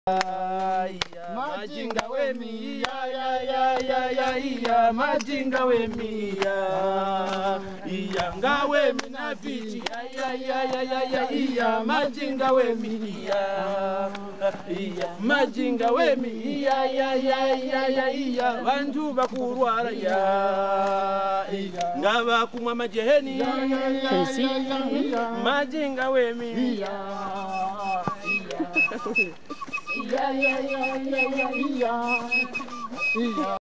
Water pump 6 - Wells for Zoe, Malawi
Northern Malawi near Mzuzu. Location: Doroba area, Dika Mshanga village